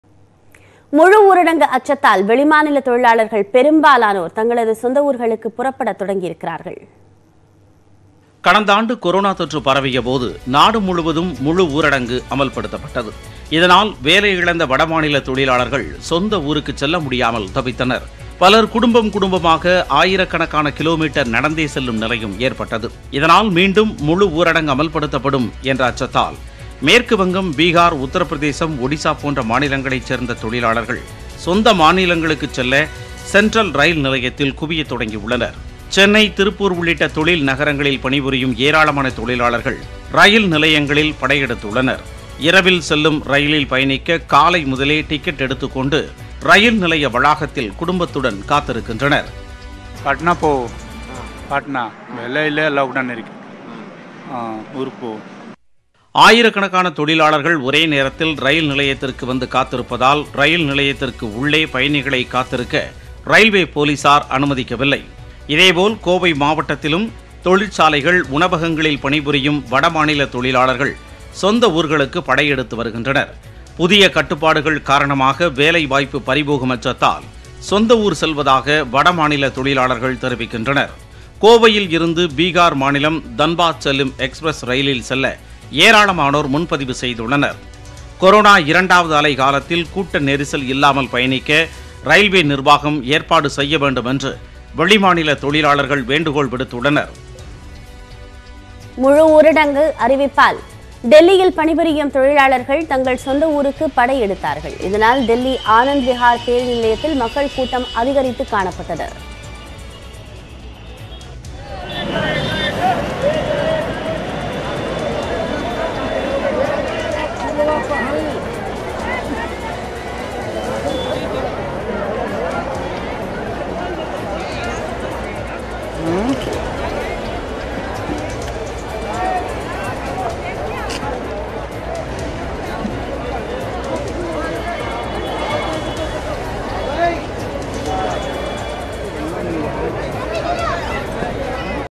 முழு ஊரடங்கு அச்சத்தால் வடமாநில தொழிலாளர்கள் பயணம் | Coronavirus Second Wave
சொந்த ஊர்களுக்கு படையெடுக்கும் தொழிலாளர்கள்... | சென்னை சென்ட்ரல் ரயில் நிலையத்தில் காத்திருப்பு | முழு ஊரடங்கு அச்சத்தால் வடமாநில தொழிலாளர்கள் பயணம் | சொந்த ஊருக்கு படையெடுக்கும் புலம்பெயர் தொழிலாளர்கள் | Coronavirus Second Wave